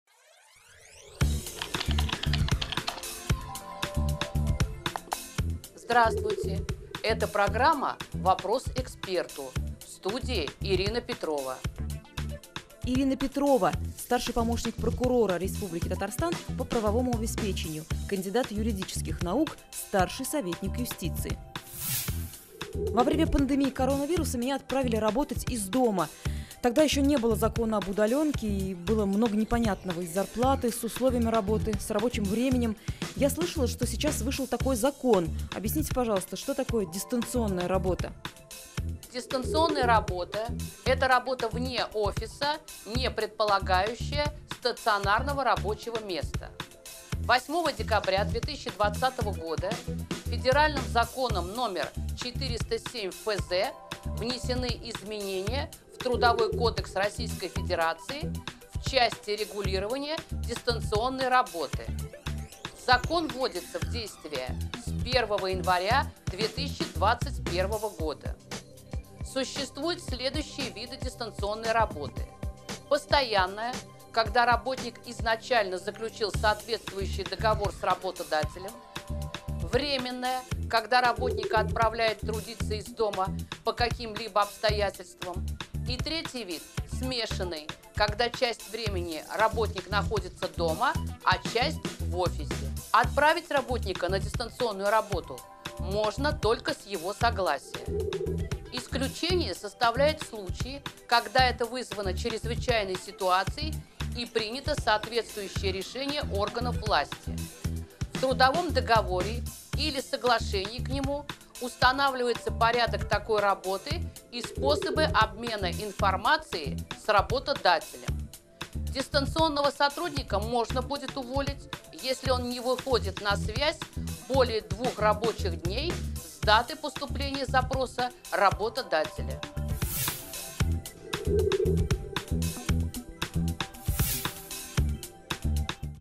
Вопрос эксперту - "Что такое удаленка?"
Вопрос:
Во время пандемии коронавируса  меня отправили работать «из дома»?  Тогда еще не было закона об удалёнке  и было много непонятного с зарплатой,  с условиями работы,  рабочим временем. Я слышала, что сейчас вышел такой закон. Объясните, пожалуйста, что такое дистанционная работа?
Отвечает:
Ирина Петрова - старший помощник прокурора Республики Татарстан по правовому обеспечению, кандидат юридических наук, старший советник юстиции
Ответ:
Дистанционная работа - эта работа вне офиса, не предполагающая стационарного рабочего места. 8 декабря 2020 года Федеральным законом №407-ФЗ внесены изменения в Трудовой кодекс РФ в части регулирования дистанционной работы. Закон вводится в действие с 1 января 2021 года. Существуют следующие виды дистанционной работы: постоянная, когда работник изначально заключил соответствующий договор с работодателем. Временная, когда работника отправляют трудиться из дома по каким-либо обстоятельствам. И третий вид - смешанный, когда часть времени работник находится дома, а часть - в офисе. Отправить работника на дистанционную работу можно только с его согласия. Исключение составляют случаи, когда это вызвано ЧС и принято соответствующее решение органов власти. В трудовом договоре или соглашением к нему устанавливается порядок такой работы и способы обмена информацией с работодателем. Дистанционного сотрудника можно будет уволить, если он не выходит на связь более 2-ух рабочих дней с даты поступления запроса работодателя.